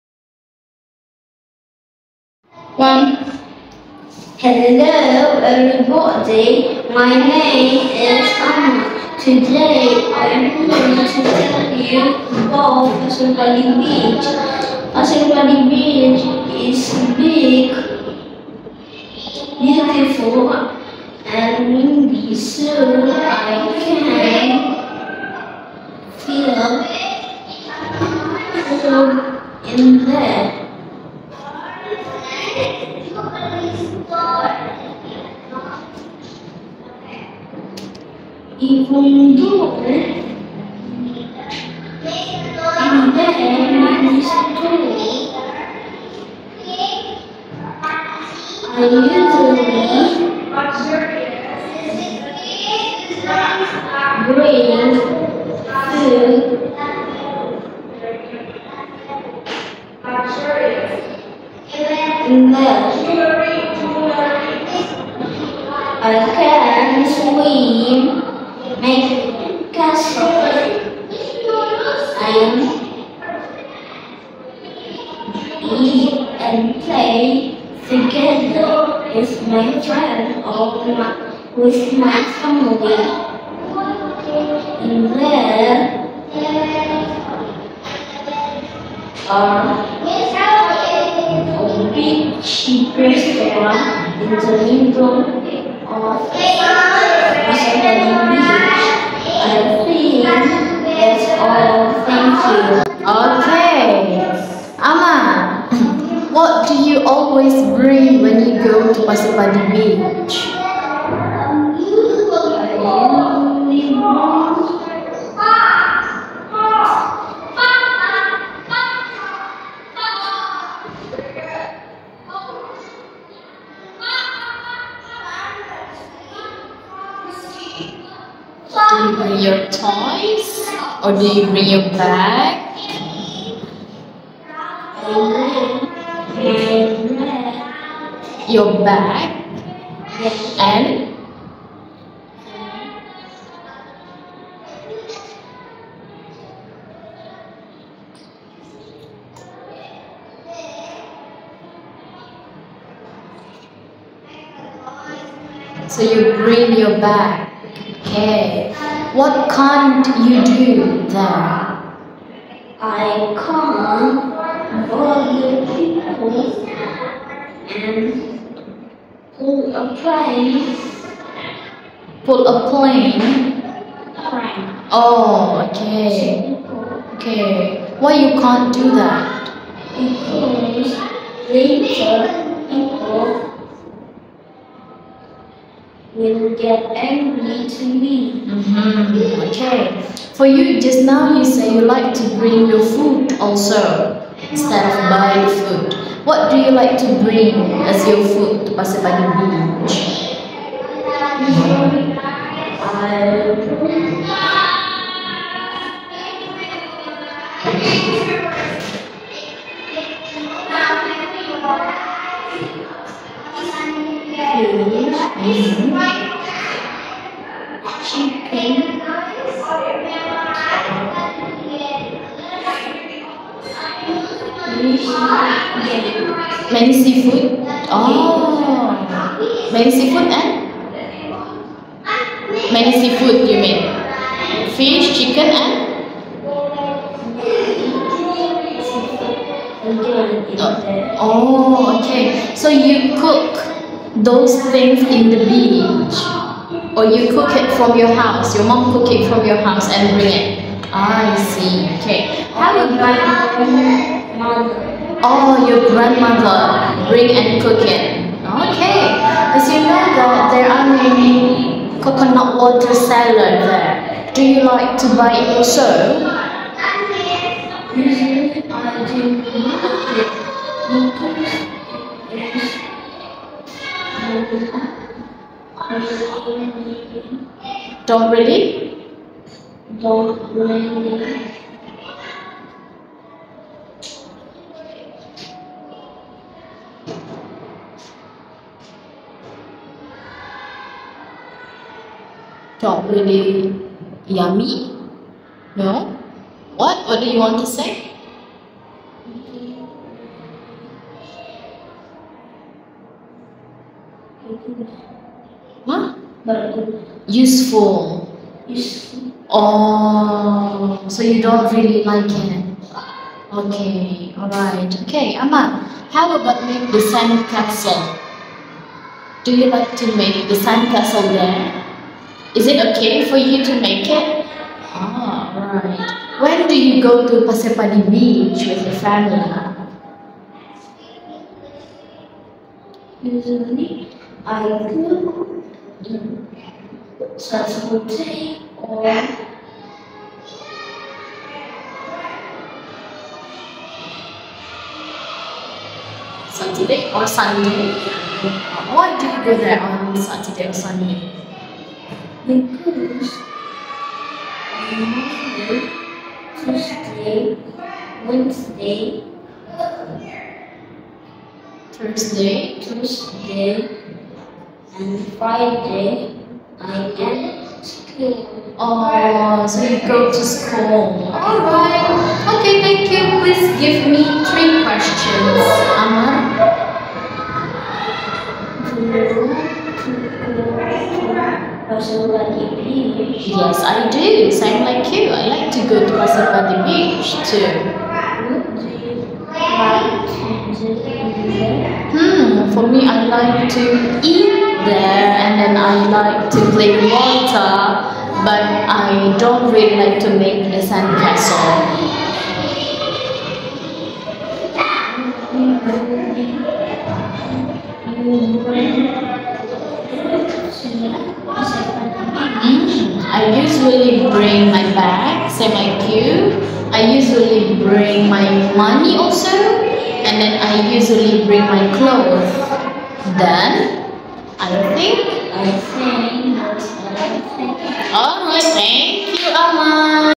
AMMAR (DARLINGTON CLASS) [FINAL ASSESSMENT: PUBLIC PLACE]
It's the final assessment of Darlington Class

Redbook for Kids (First Level): Chapter 4 (Public Place)
The assessor (Ms. Tiara) has assessed the students in Darlington Class.
The assessment consists of 2 parts:
1) Describing their favourite public place
2) Answering and giving questions